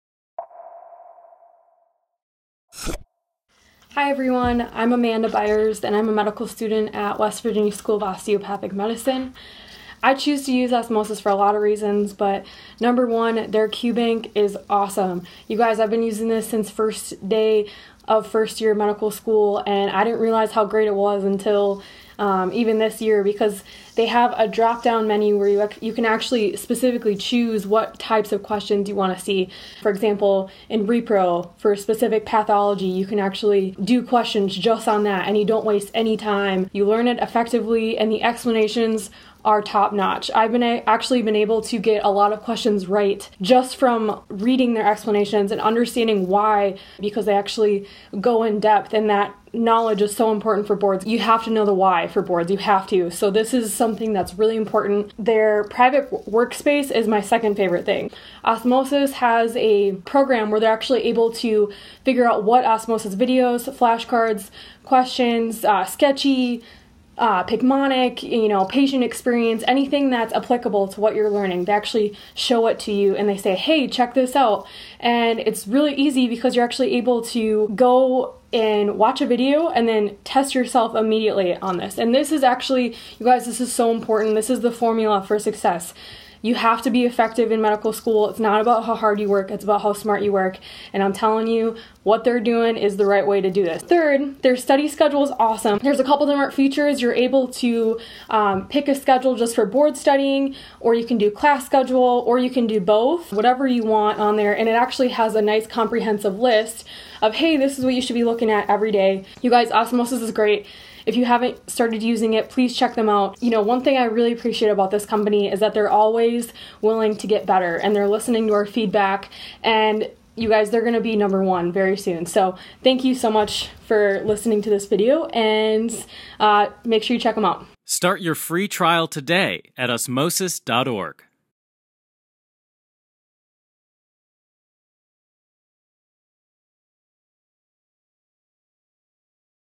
Osmosis.org at West Virginia School of Osteophathic Medicine
Amanda from WVSOM talks about her Osmosis experience! Find our full video library only on Osmosis